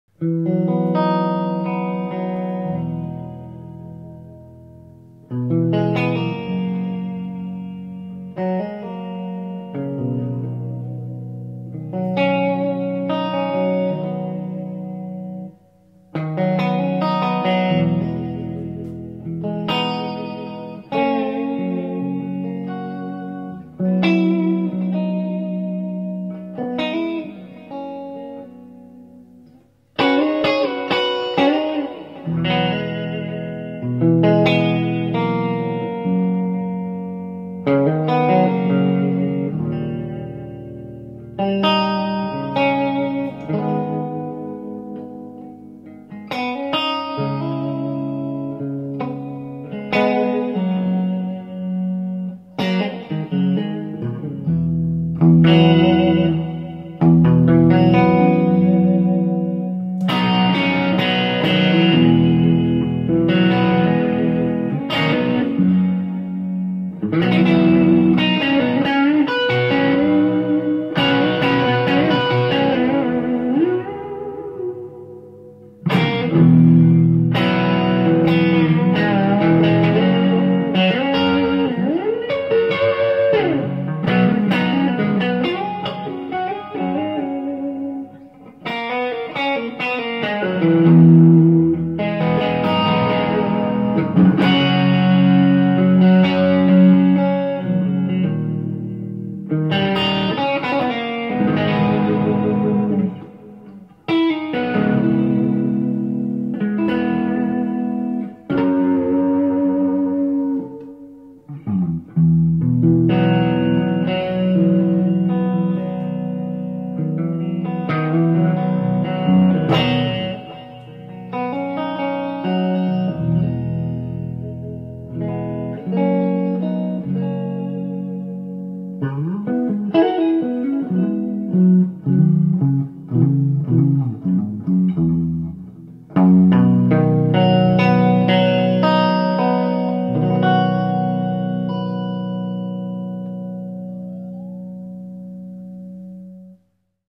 Gretsch 6122-II | 1991 | Country-classic II at The Fellowship of Acoustics
This recording was made using:

- 1 neumann KM-184 as room mic
- 1 SM-58 cab mic
- peavey classic 50 (4x10)
- Erwins Pedalboard
- Zoom H4n